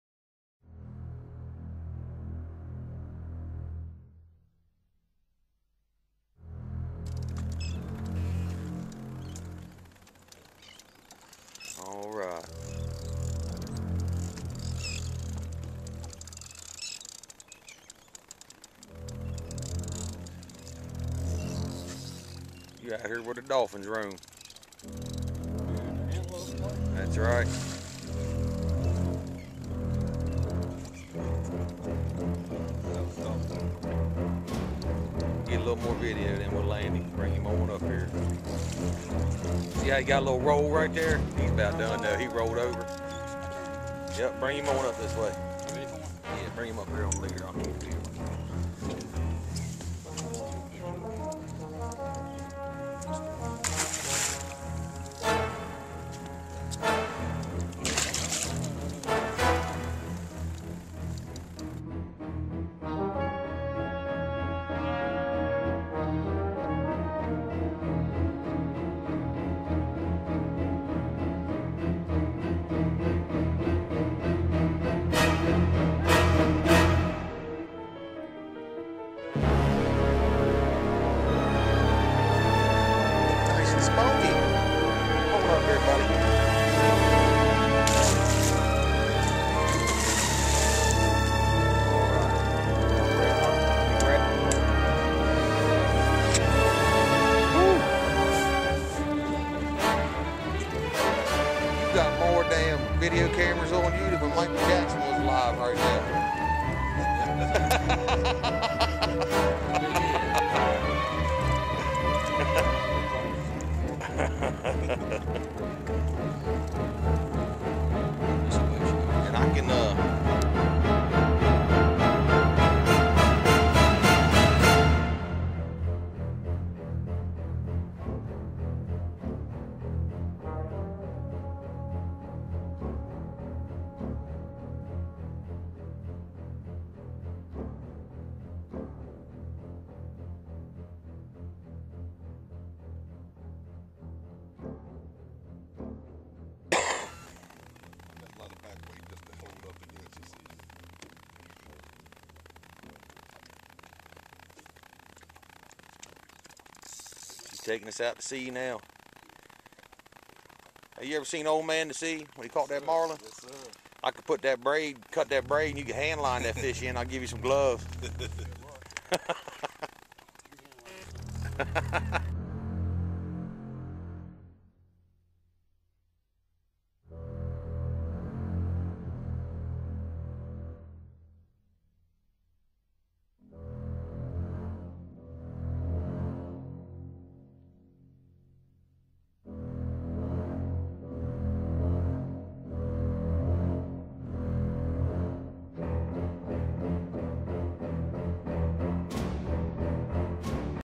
Tarpon Fishing in North Inlet at DeBordieu Beach South Carolina
Went 2/2 on the tarpon fishing with Barrier Island Guide Service in Georgetown South Carolina. Providing Tarpon guided trips and charters in the Myrtle Beach and Georgetown areas.